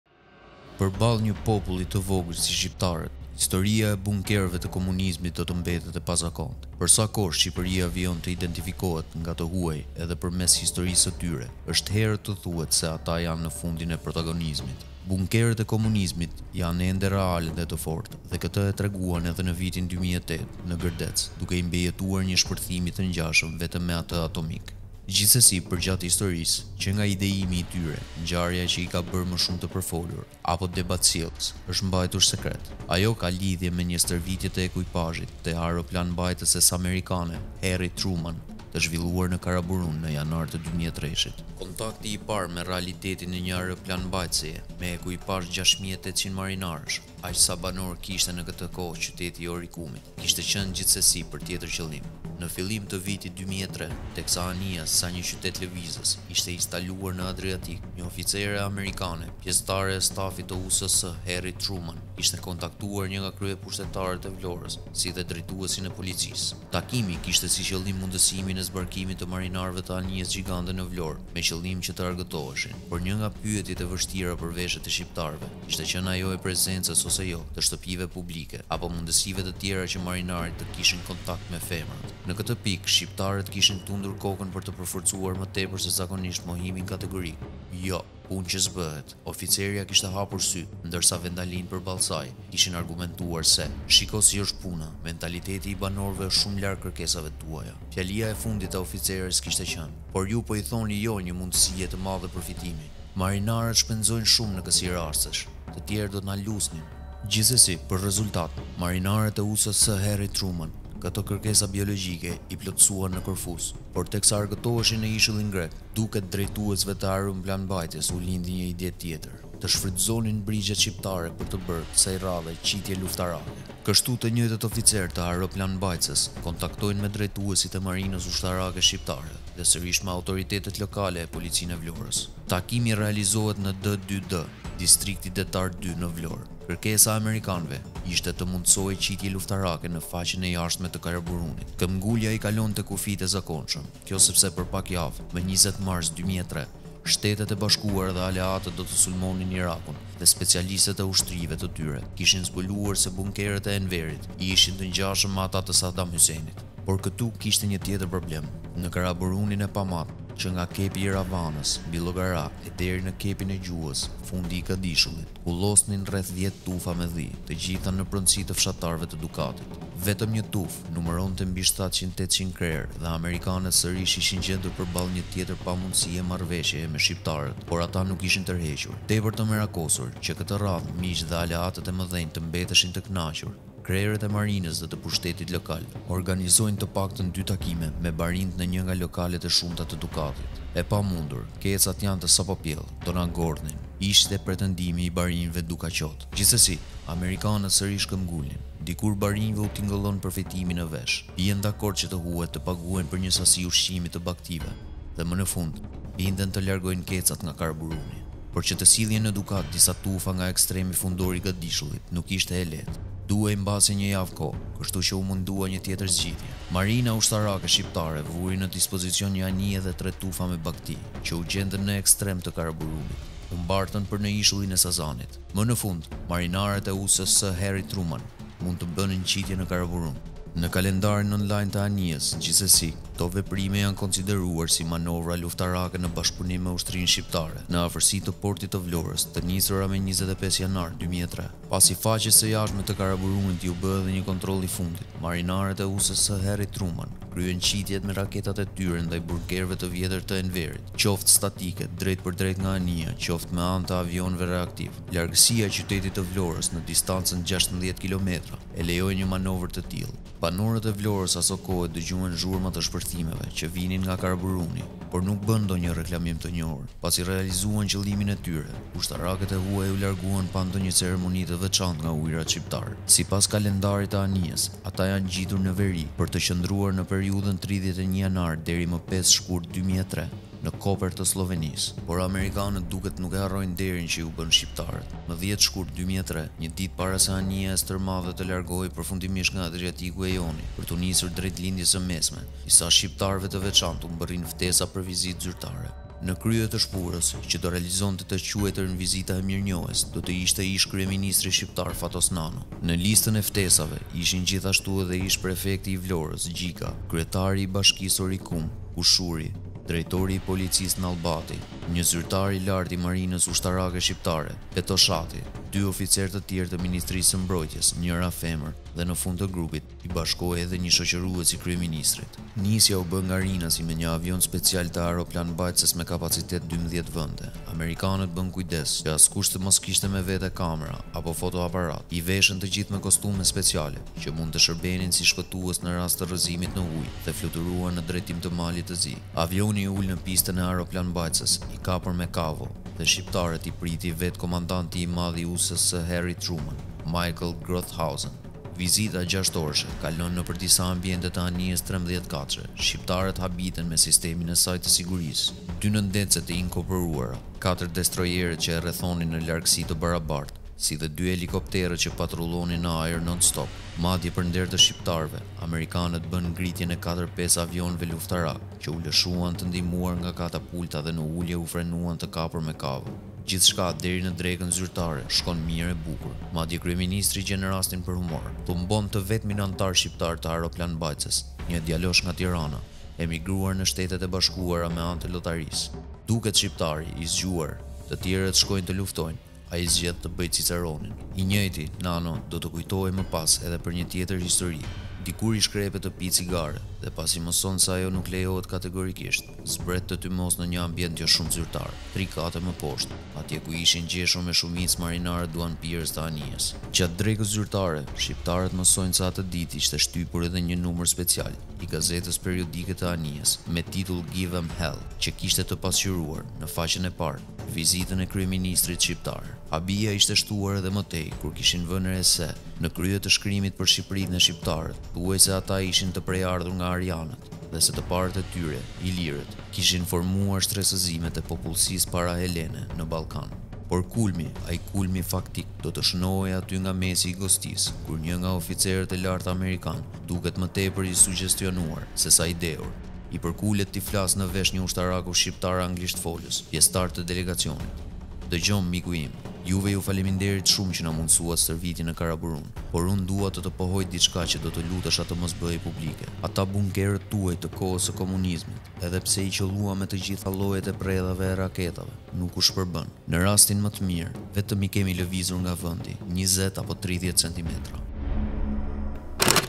“Beteja” e panjohur e 2003, kur bunkerët e Enverit sfiduan raketat e NATO-s/ Nga Ferdinand Dervishi
Një histori e pazakontë, që i ka dhënë një përgjigje pikëpyetjes së madhe që mundonte dikur komunistët shqiptarë: Bombat e NATO-s, apo të imperialistëve amerikanë, a do ti vrisnin nëse do të strehoheshin brenda bunkerëve?